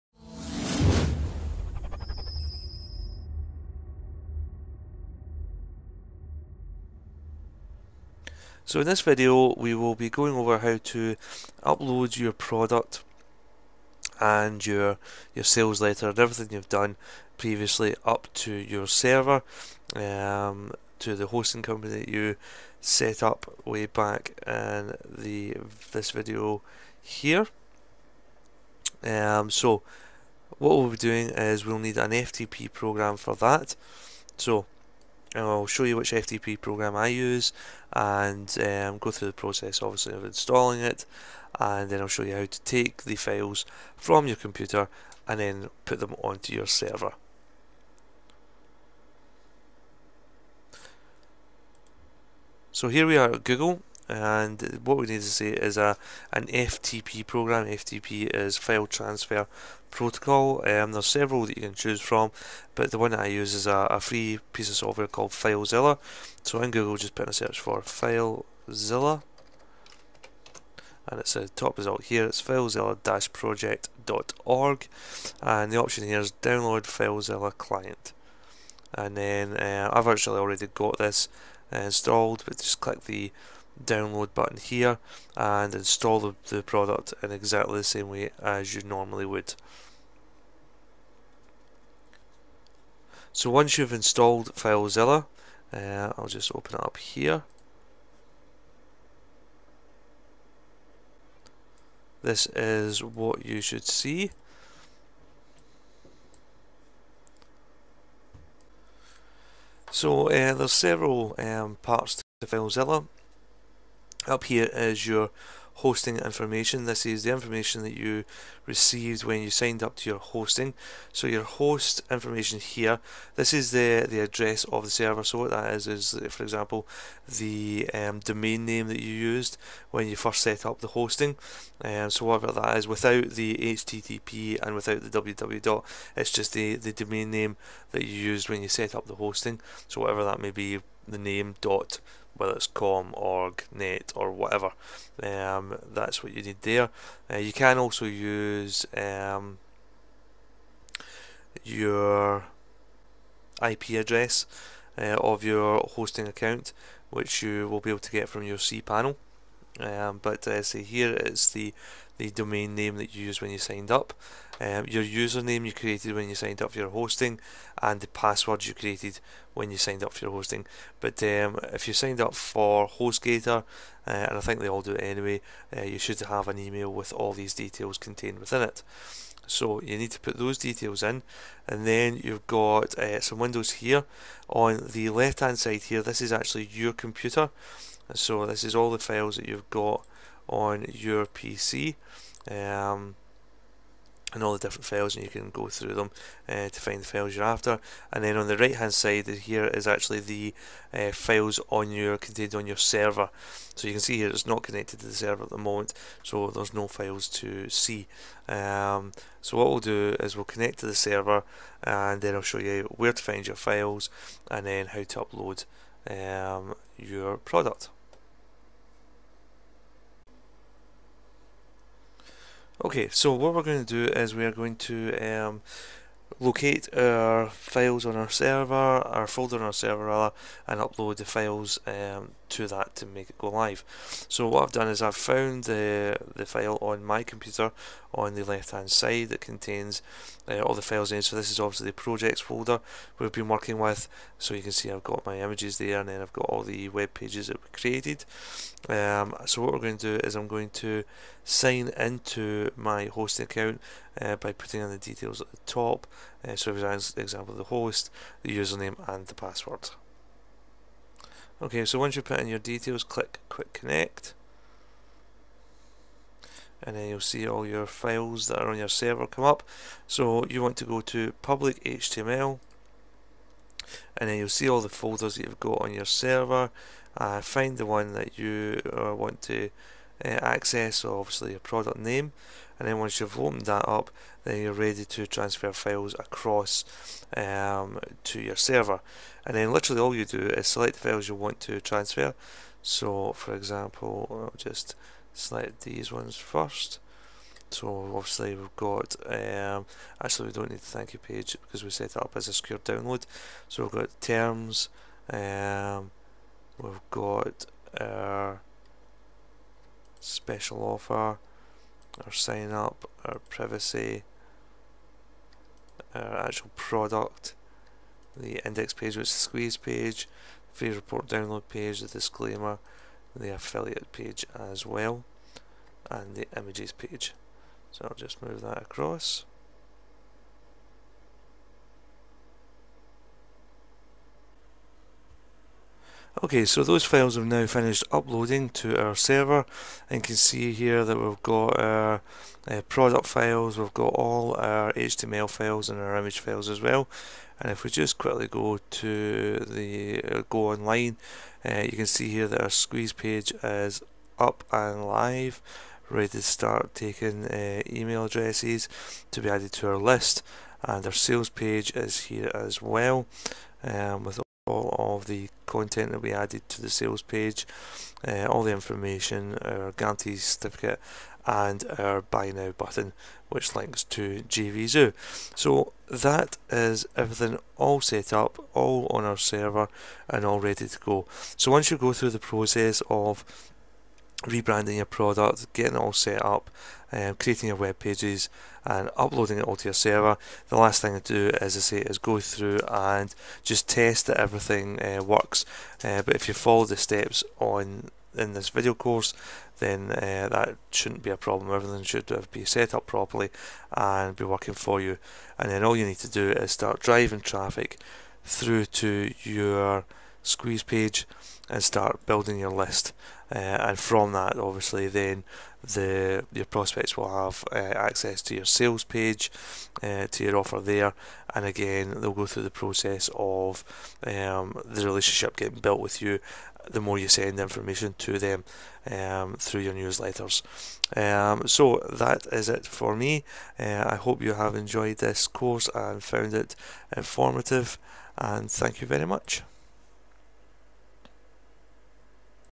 13- PLR Profits Video Course - "Uploading Your Files"
Now that we have re-branded our PLR products, Graphics and Sales pages and set up
our marketing funnel the last step is to upload everything to your server to go live!